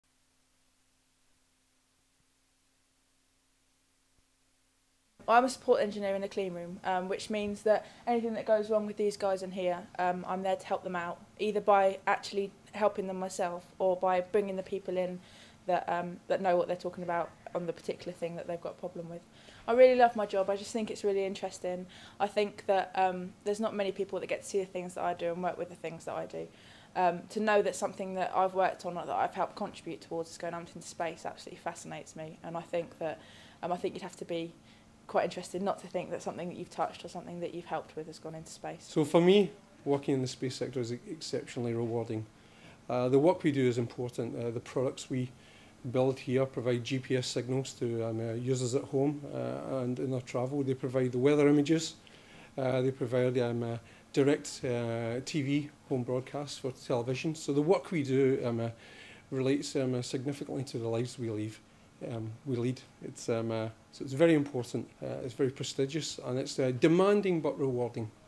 Careers in space
Staff from Astrium's Portsmouth facility talk about their exciting roles in the UK space industry.